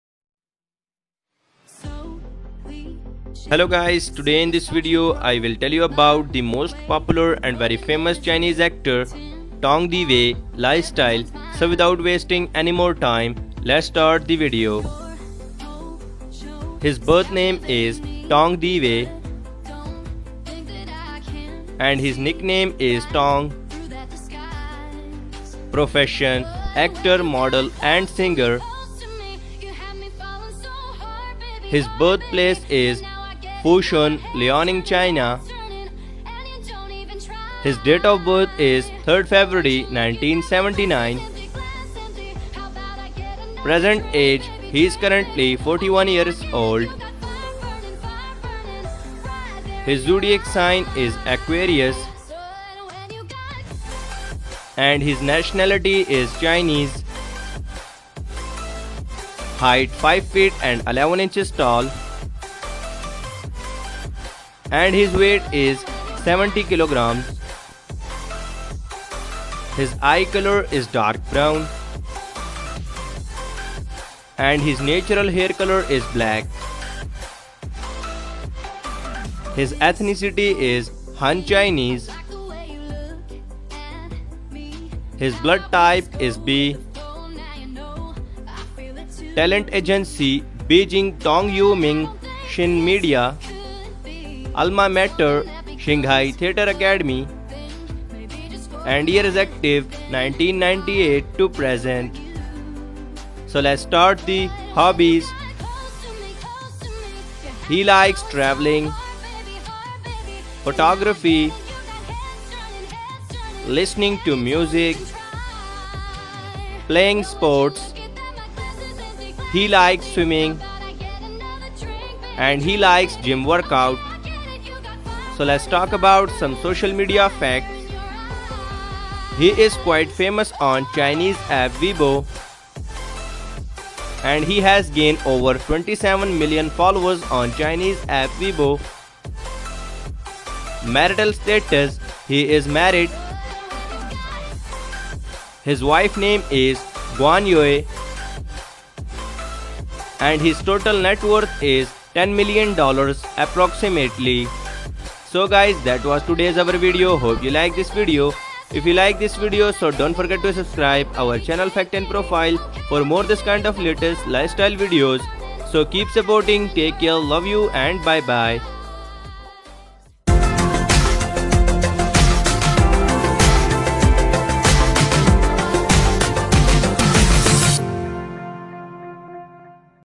Tong Da Wei (Secret Keepers 2020) Lifestyle, Networth, Age, Girlfriend, Income, Hobbies, & More.
The Most Popular And Most Handsome Chinese Actor Tong Da Wei Lifestyle Video 2020.